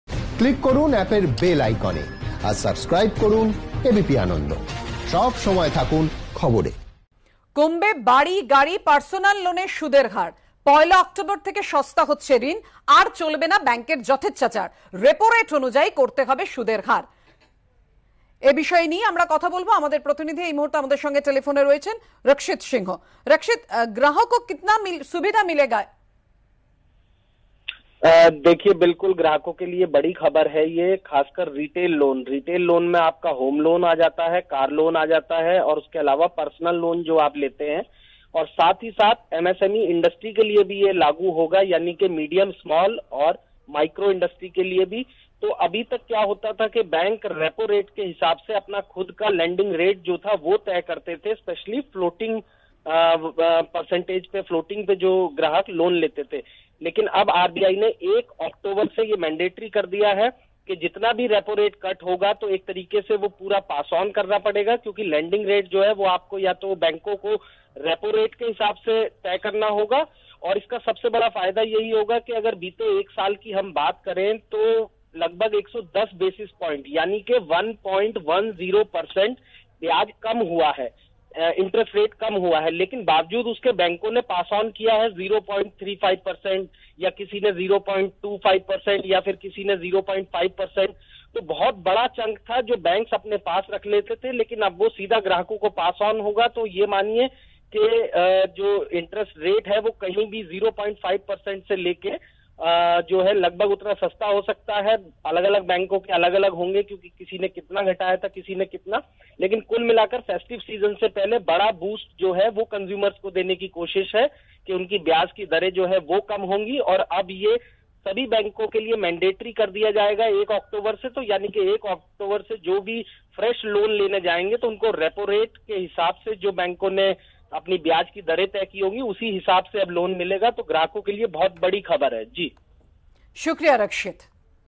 রেপো রেট অনুযায়ী করতে হবে সুদের হার, ব্যাঙ্কগুলিকে নির্দেশ আরবিআইয়ের | ABP ANANDA
কমবে বাড়ি, গাড়ি, পার্সোনাল লোনে সুদের হার। ১ অক্টোবর থেকে সস্তা হচ্ছে ঋণ। আর চলবে না ব্যাঙ্কের যথেচ্ছাচার। ‘রেপো রেট অনুযায়ী করতে হবে সুদের হার। প্রতি তিনমাসে অন্তত একবার বদলাবে সুদের হার’, সমস্ত ব্যাঙ্ককে নির্দেশ রিজার্ভ ব্যাঙ্কের। চলতি বছরেই চারবারে ১.১ শতাংশ কমেছে রেপো রেট। কিন্তু সেই তুলনায় সুদ কমায়নি ব্যাঙ্কগুলি।